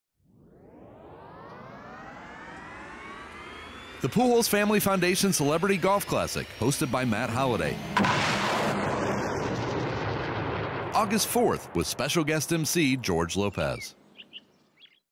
Pujols Family Foundation - 2014 Matt Holliday Golf Teaser
The following is a teaser for the 2014 Pujols Family Foundation Celebrity Golf Classic hosted by Matt Holliday. Our special guest dinner MC for this year is George Lopez.

Thank you Antidote Media Solutions for this great video.